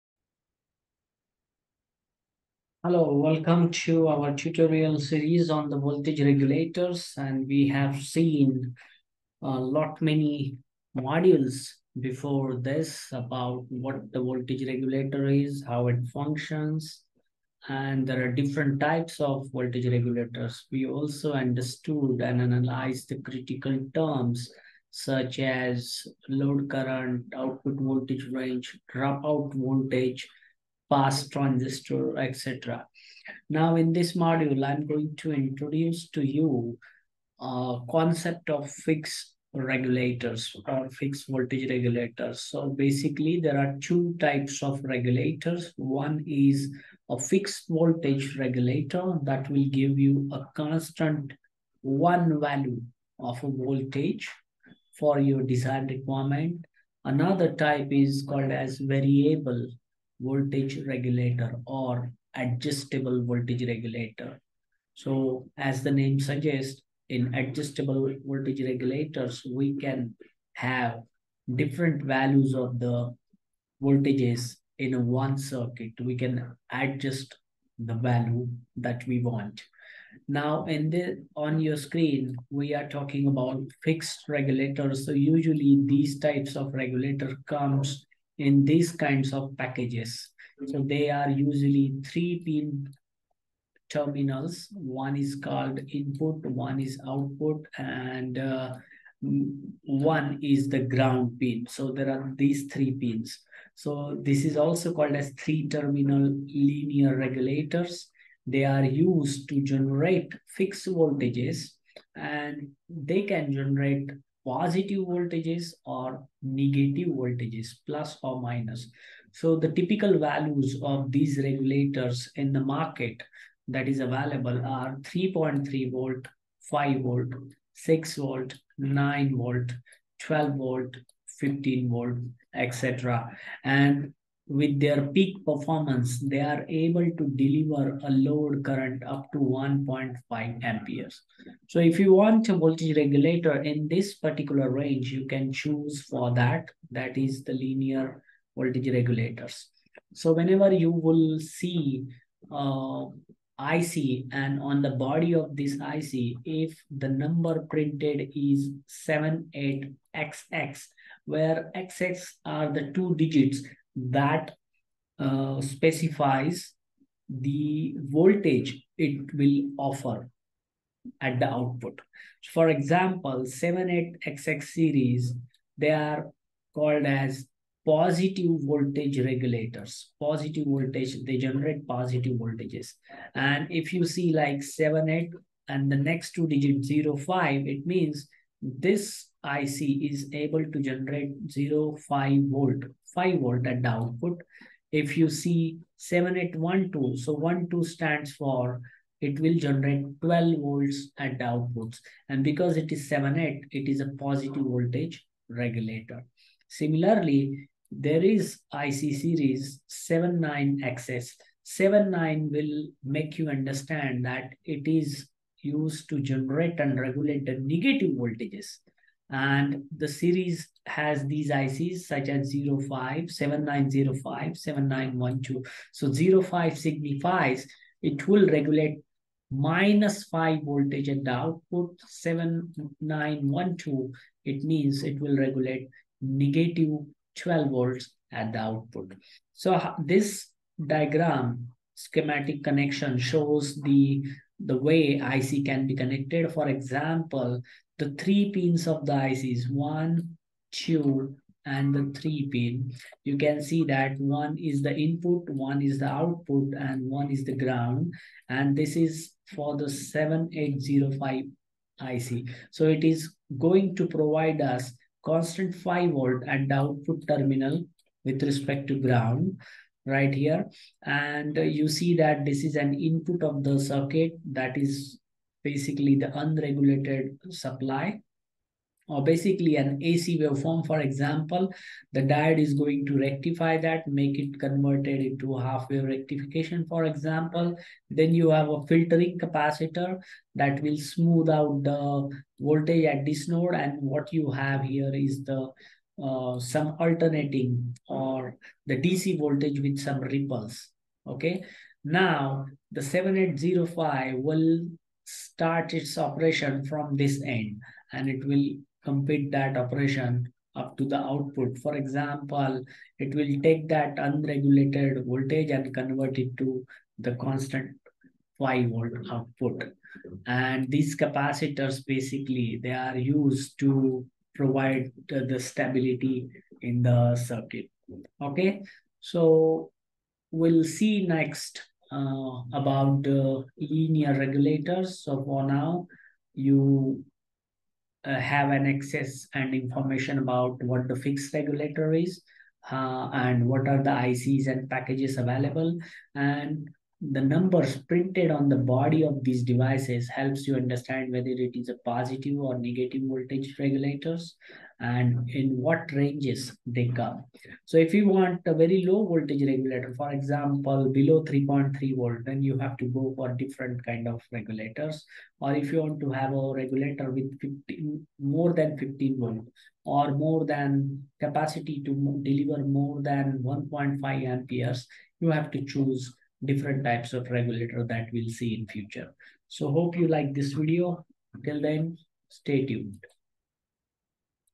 Three-Terminal Linear Regulators | The 78xx and 79xx Series Explained
Three-terminal linear regulators
used for generating "fixed" voltages.
They can generate plus or minus 3.3 V, 5 V, 6 V, 9 V, 12 V, or 15 V
for around a load of 1.5 Amperes.
The "78xx" series (7805, 7812, etc.) regulate positive voltages
The "79xx" series (7905, 7912, etc.) regulate negative voltages.

🔧 Dive into the world of fixed voltage regulators with the iconic "78xx" and "79xx" series! In this video, we explore the capabilities and applications of these three-terminal linear regulators that can generate fixed voltages, ranging from plus or minus 3.3 V to 15 V. Whether you're a beginner or a seasoned electronics enthusiast, join us on this journey to understand how these regulators work and where they shine.

Key Topics Explored:

Introduction to Fixed Voltage Regulators:

What are three-terminal linear regulators, and how do they differ from other voltage regulation methods?
The "78xx" Series: Positive Voltage Regulators:

Unravel the secrets behind the "78xx" series, such as 7805 (5 V), 7812 (12 V), and others.
Explore how they maintain a steady positive output voltage under varying loads.
The "79xx" Series: Negative Voltage Regulators:

Delve into the realm of negative voltage regulation with the "79xx" series, including 7905 (minus 5 V), 7912 (minus 12 V), and more.
Understand how they provide a stable negative output voltage.
Output Voltage Options:

Discover the wide range of fixed output voltages available, from 3.3 V to 15 V, catering to various electronic applications.
Load Handling Capacity:

Explore the capabilities of these regulators to deliver a reliable power supply for loads up to 1.5 Amperes.
Applications and Use Cases:

Real-world scenarios where the 78xx and 79xx series shine, and tips on selecting the right regulator for your project.
Best Practices and Considerations:

Practical advice on heat dissipation, capacitor usage, and other considerations for optimal performance.
Whether you're designing a power supply for your latest project or simply curious about these ubiquitous voltage regulators, this video has something for everyone.

Let's unravel the mysteries of the 78xx and 79xx series together! 💡🔍